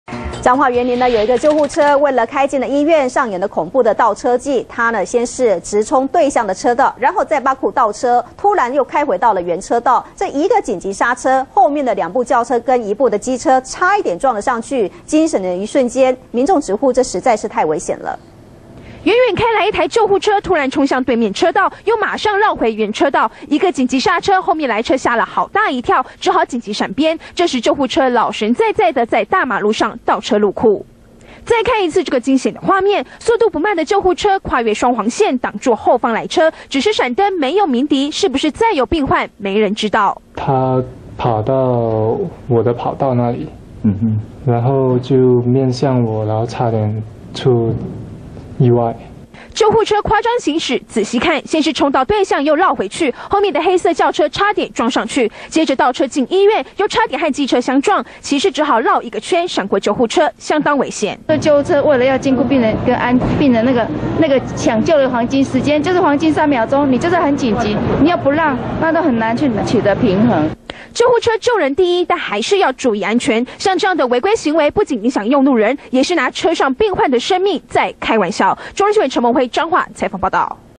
救護車未鳴笛衝對向 急剎倒車險釀撞
中天新聞》彰化員林一輛救護車為了開進醫院，在街頭上演恐怖的倒車技，先是直衝對向車道再倒車，突然又開回原車道，接著又緊急剎車，使得後面的兩部轎車和一輛機車差點撞上救護車，讓民眾直呼太危險。